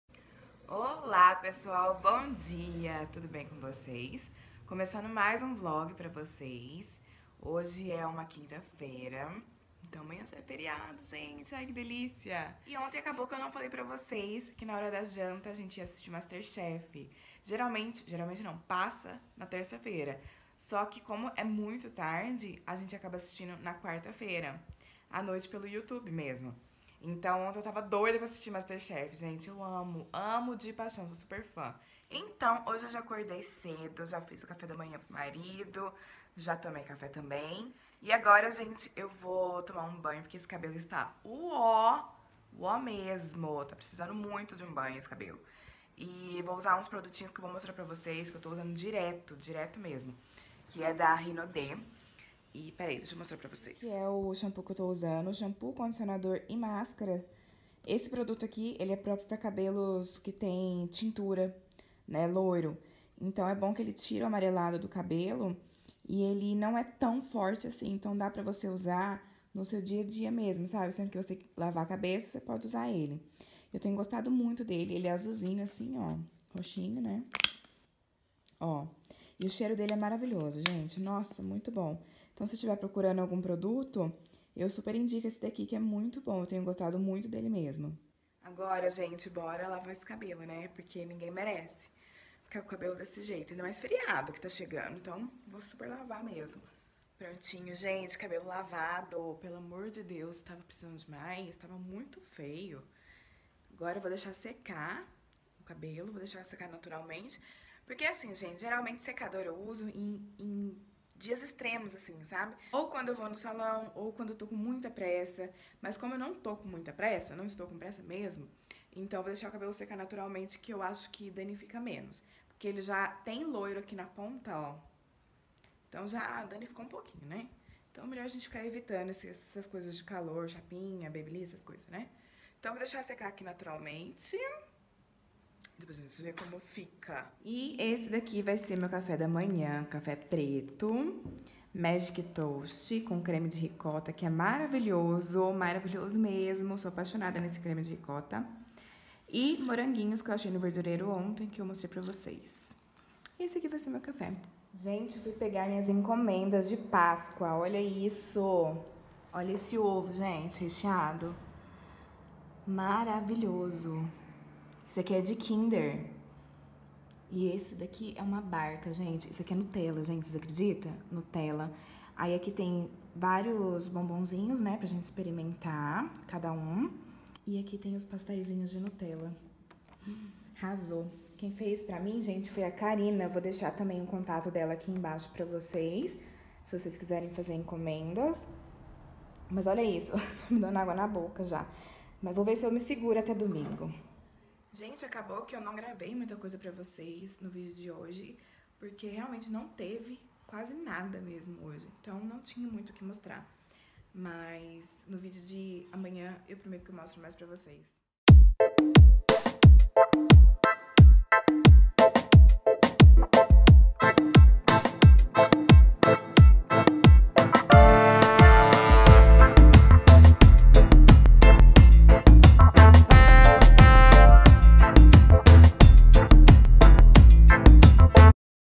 Vlog todo dia! Dia 5 - por Ana Maria Guimarães
Oi oi Pessoal, tudo bem?
Mais um dia de vlog para vocês!
Com direito a ovo de páscoa de colher maravilhosoooo

Bora conferir?!

🔔 Vídeos novos: Quinta e Domingo
🎁 Caixa Postal 14, CEP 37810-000, Guaranésia - MG